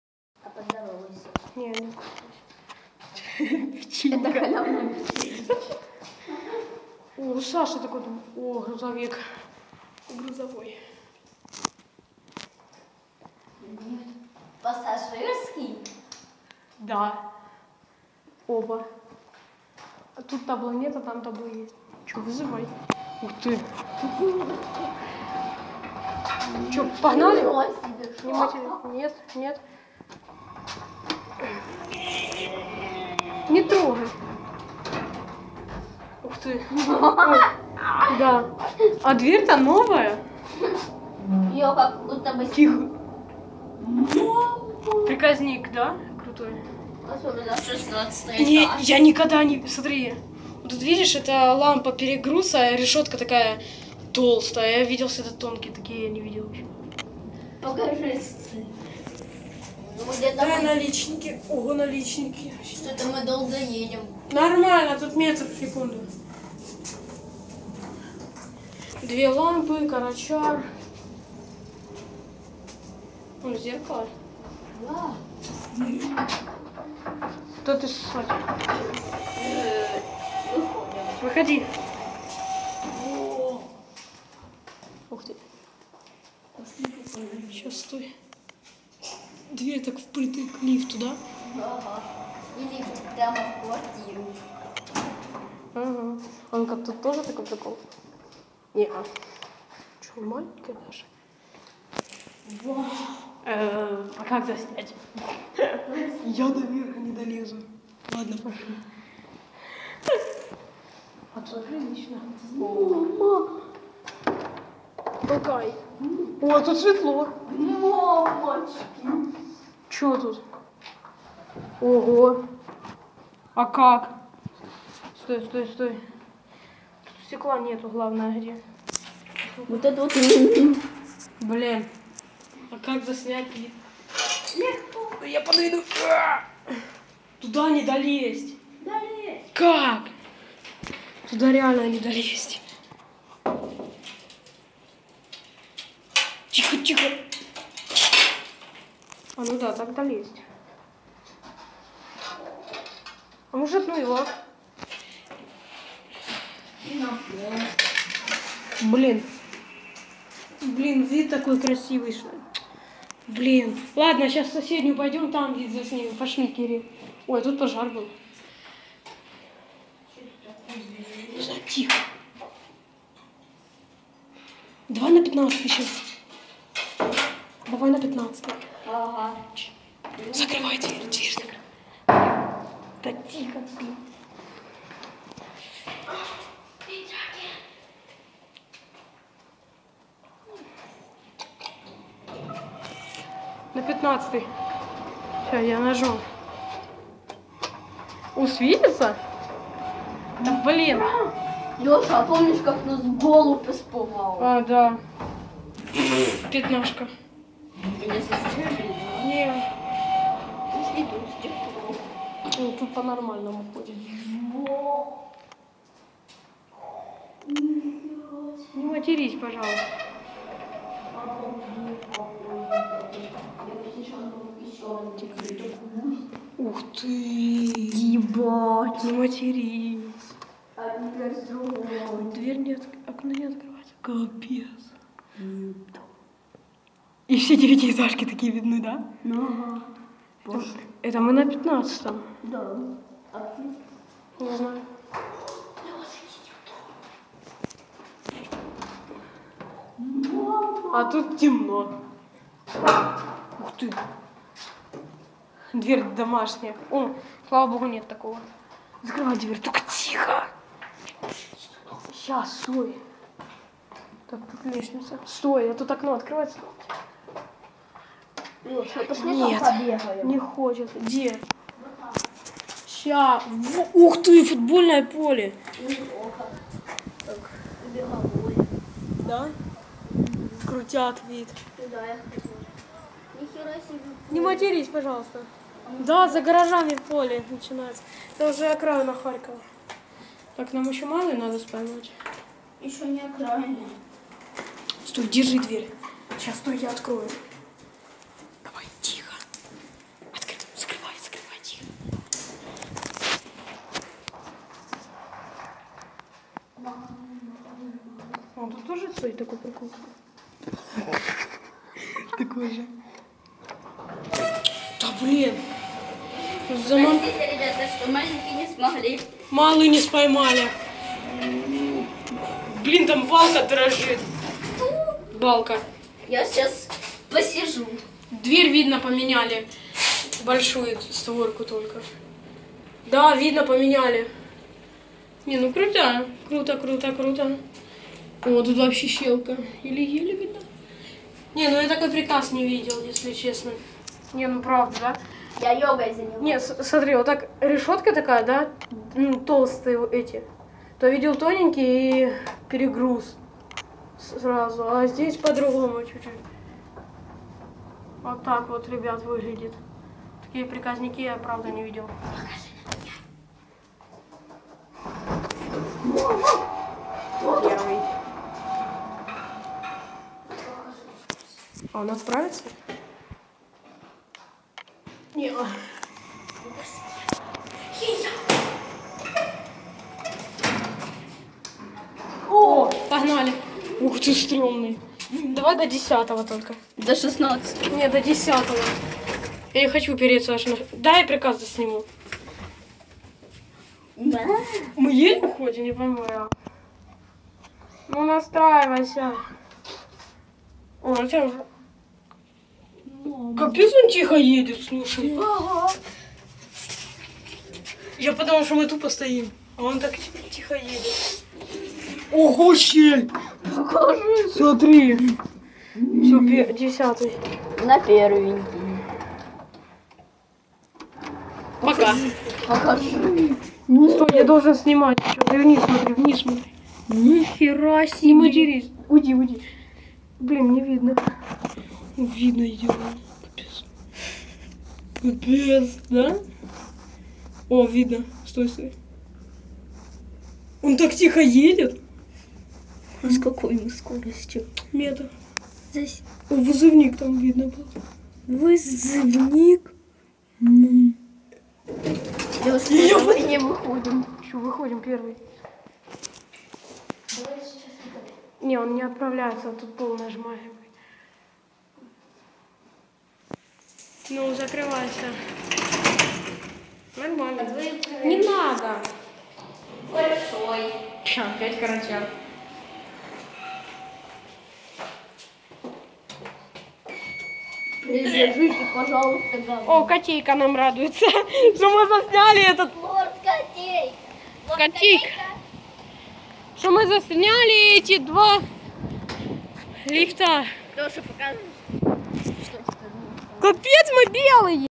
(г. Харьков Лифт МЛЗ и КМЗ 80х г/п 320 и 500 кг скорость 1.0 м/с(54)
грузопассажирский и пассажирский

г. Харьков, Ж/М Роганский, 1-й котласский пр-д 22

м. Харків, Ж/М Роганський, 1 котласький проїзд 22